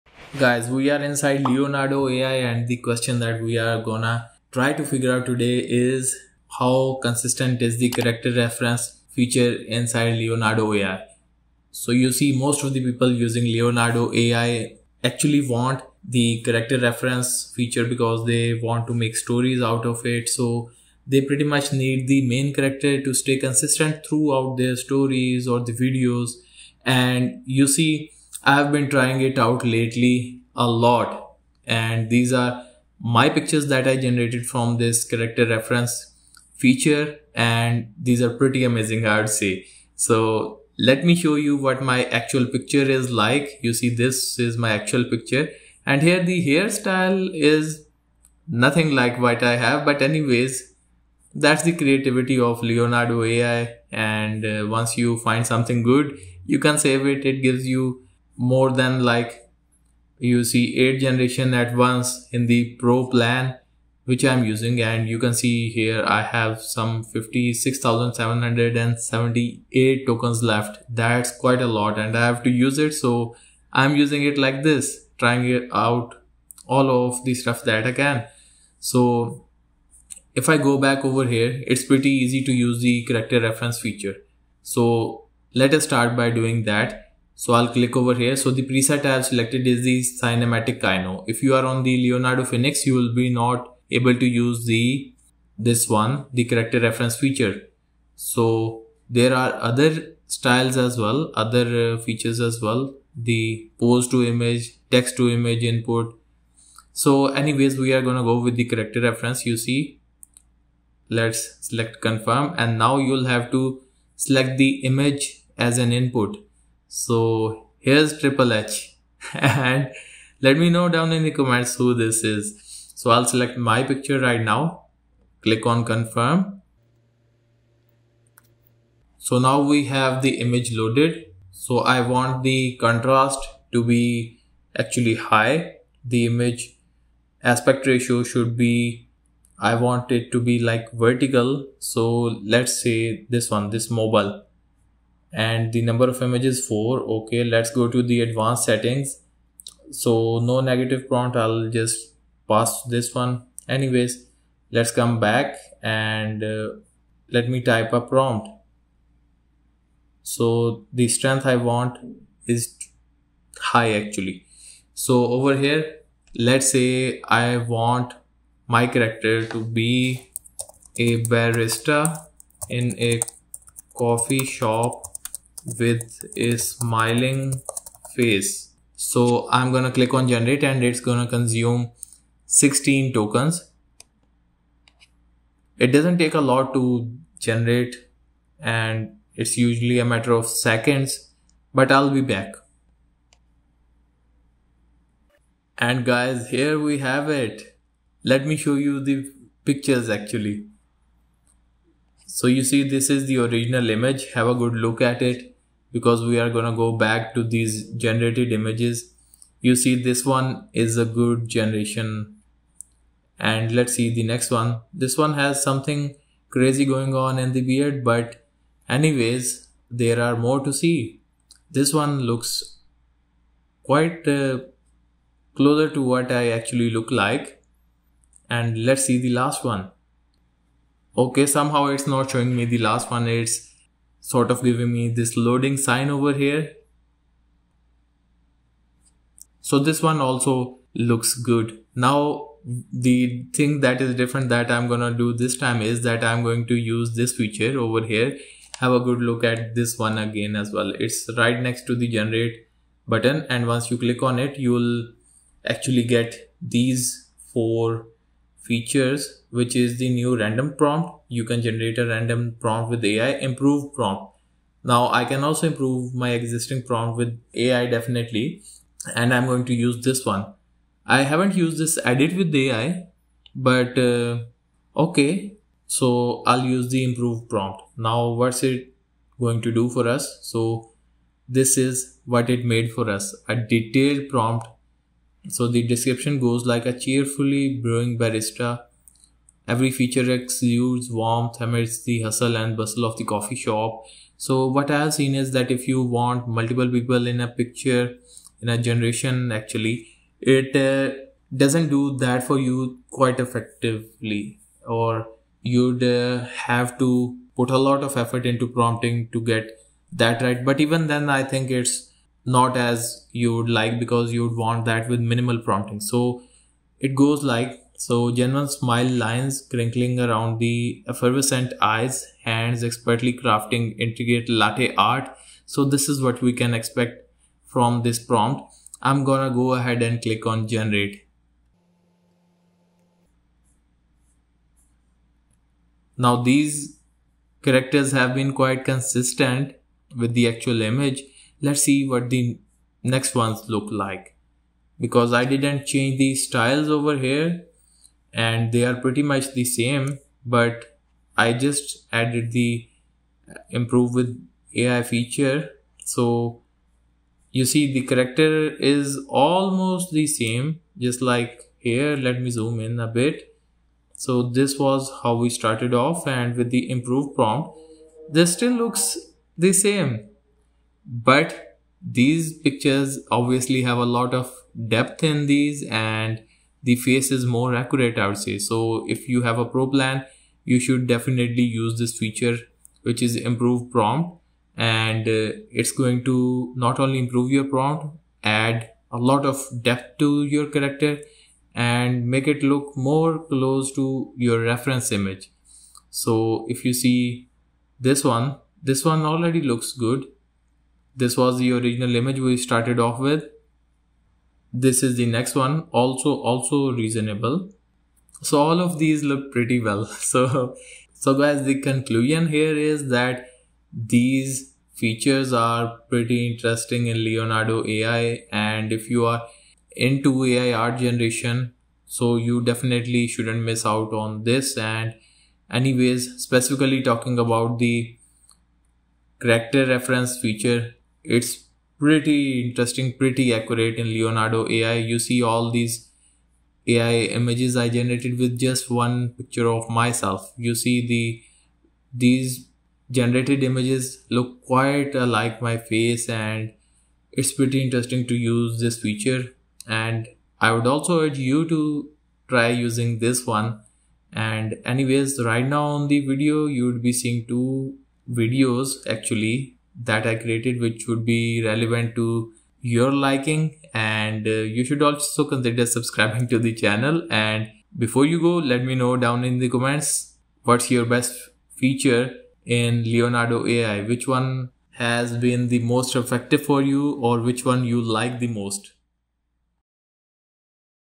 How Consistent is Character Reference in Leonardo AI Image Generations
Leonardo AI is one of the best tools out there for not only generating AI images but also making consistent characters with its content reference feature. That makes it stand from other AI Art Gneration tools out in the world of AI.

In thi video i hope you would get a good idea of how you can keep characters consistent without doing much efforts.

Dont forget to watch the other videos on my channel AI Matters Media if you are interested in AI art and news.